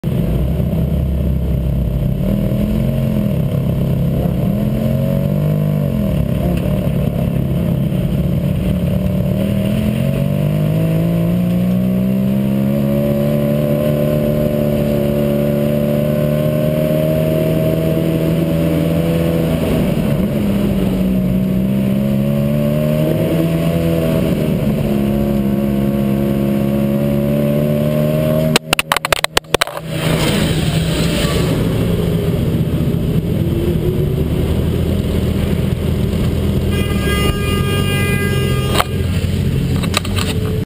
London supercar sunday 6Feb11 - Vid1
At last a perfect time to get footage of my Gallardo. What could possably go wrong ?